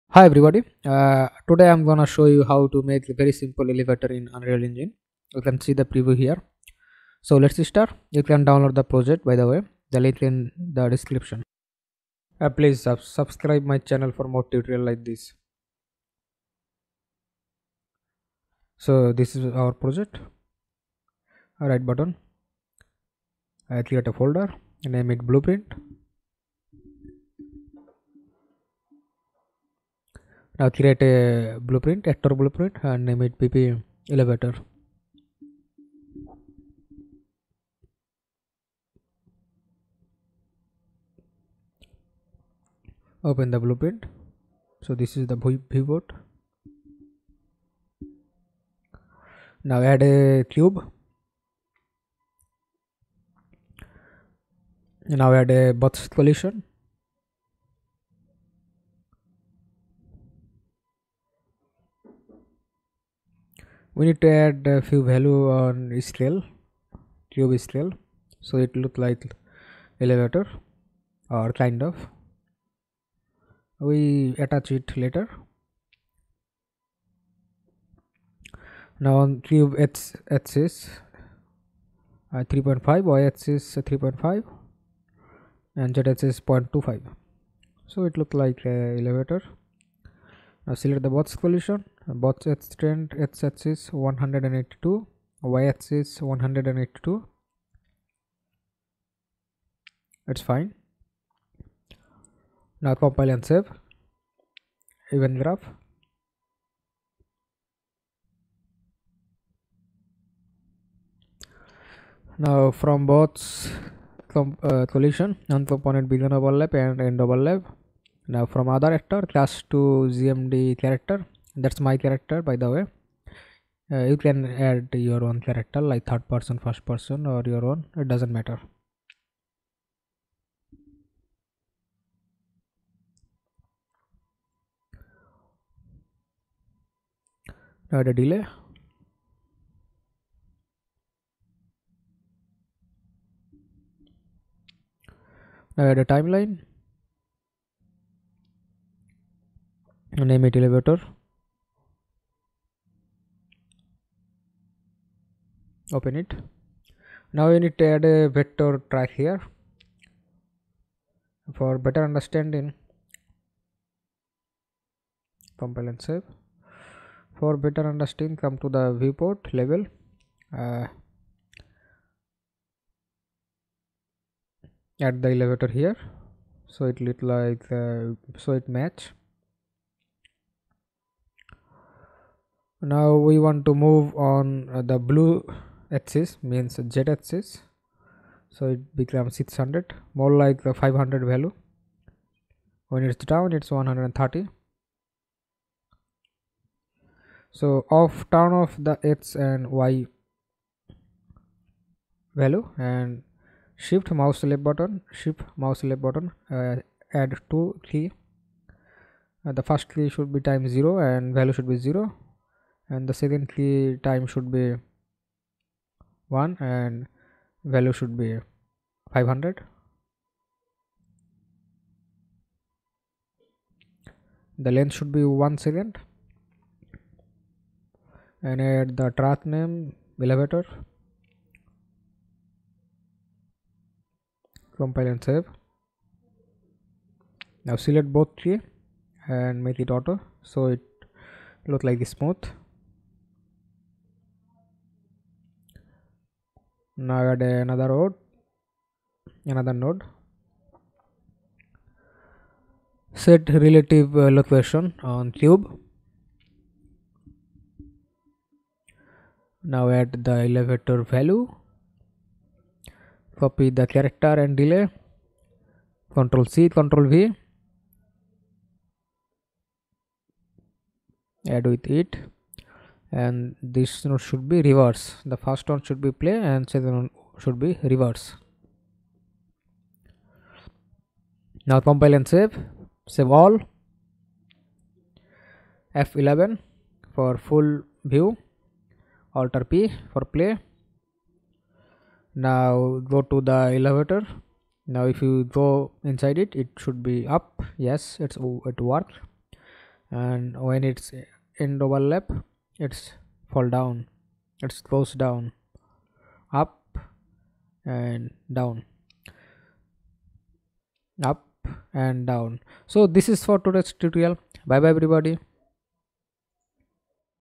Make Simple Elevator in Unreal Engine 5
Hi everybody, Today I am gonna show you how to make very simple elevator in unreal engine. You can see the preview here. So Let's start. You can download the Project from below link